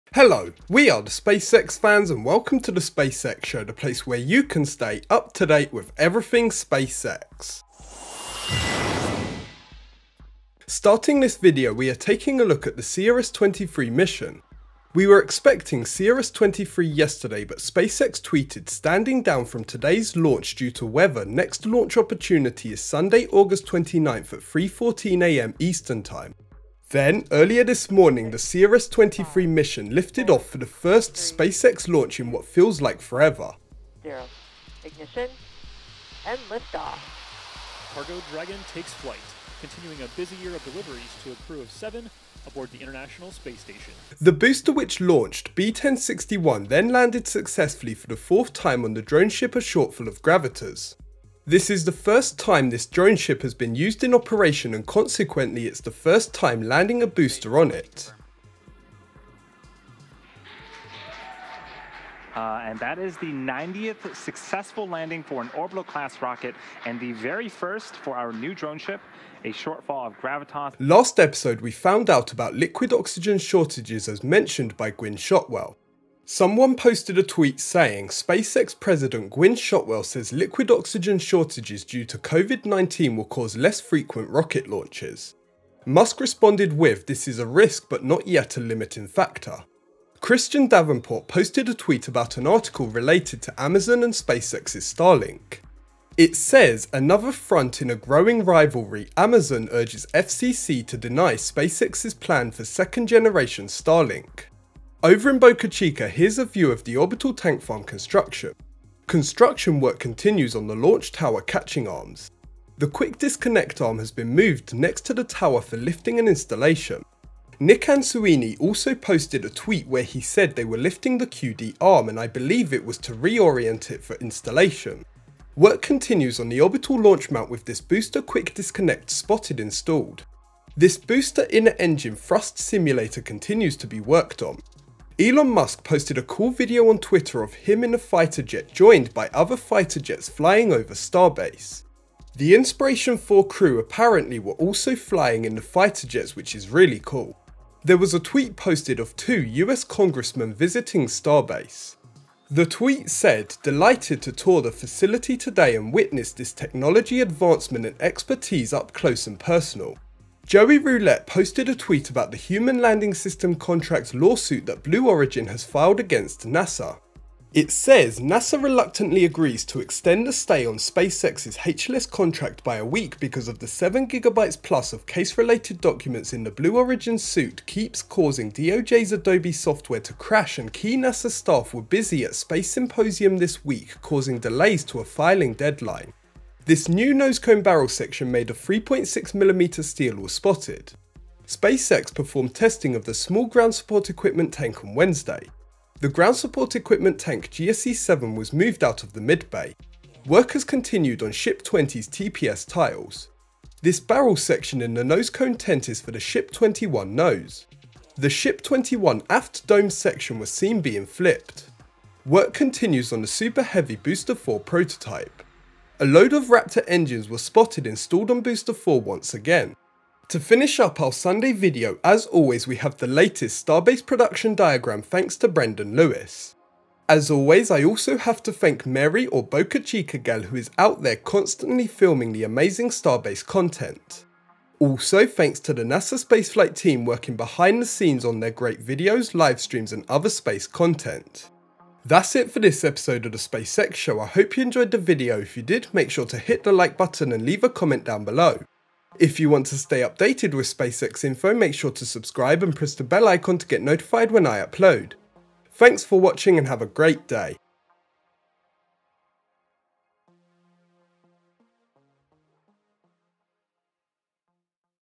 SpaceX Starship Updates! Small Ground Support Equipment Test Tank Tested! TheSpaceXShow
Welcome to the latest SpaceX Starship updates video from TheSpaceXFans. Finally, after what feels like forever, we have another SpaceX launch with the CRS-23 mission as well as other standard SpaceX news. Then, over in Boca Chica, work continues to progress rapidly with the orbital infrastructure at Starbase as they look to install the tower quick disconnect arm soon. Then, whilst I thought we would have seen the first Starship testing by now, there was instead the small ground support equipment test tank tested! Of course, work is continuing on Starship 20 in preparation for the first Ship 20 testing, as well as work on Super Heavy Booster 4 in preparation for testing of that too. Sit back and enjoy!

Mary (BocaChicaGal). More Raptor Engines Installed on Booster 4 | SpaceX Boca Chica. YouTube. NASASpaceflight. August 25, 2021.

Mary (BocaChicaGal) & NSF Robotic Camera Team. Starship Quick Disconnect Arm Moved Next to Launch Tower Ahead of Installation | SpaceX Boca Chica. YouTube. NASASpaceflight. August 28, 2021.

Mary (BocaChicaGal). Super Heavy Booster Quick Disconnect Installed on Orbital Launch Mount | SpaceX Boca Chica. YouTube. NASASpaceflight. August 27, 2021.

Mary (BocaChicaGal) & NSF Robotic Camera Team. GSE Test Tank Undergoes Cryogenic Proof Testing | SpaceX Boca Chica. YouTube. NASASpaceflight. August 26, 2021.

Brendan Lewis. Starbase Production Diagram. Twitter. Brendan Lewis. August 28, 2021.